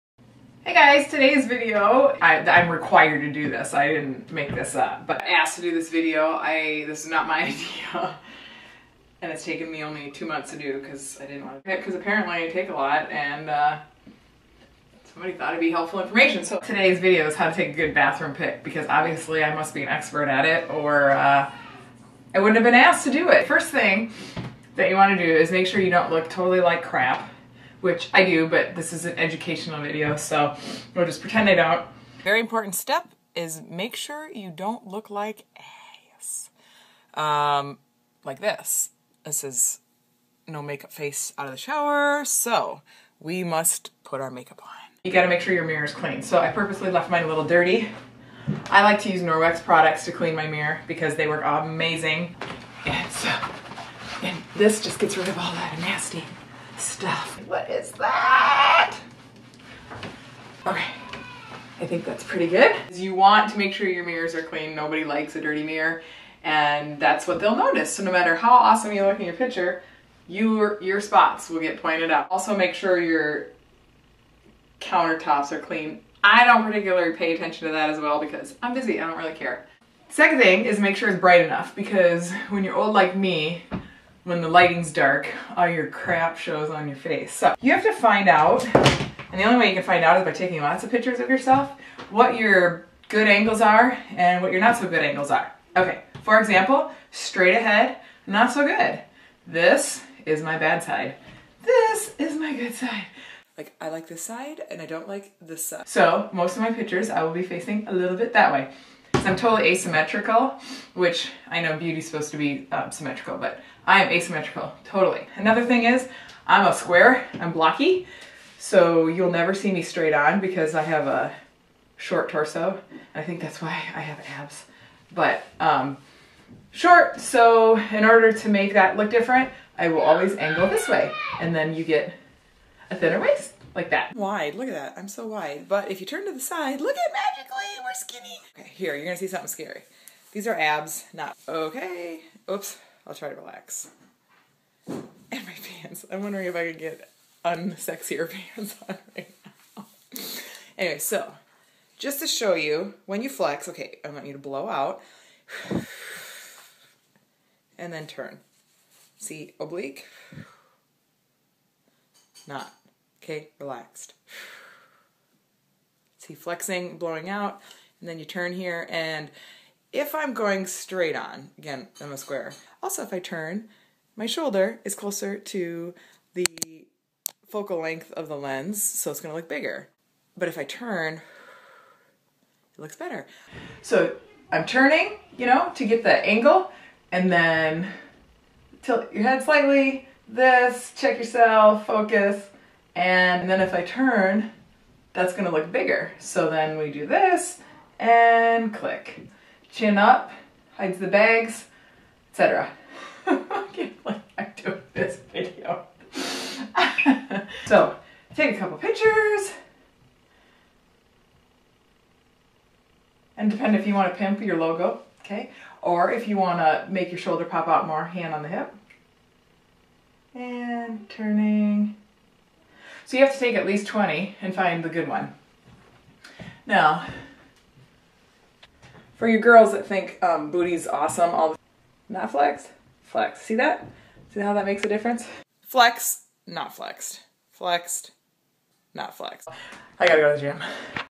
Fitness model shows how to take a good bathroom pic
I made this video  a few weeks ago and then of course other people tried to duplicate, but here is the first how to take a good pic in the bathroom lol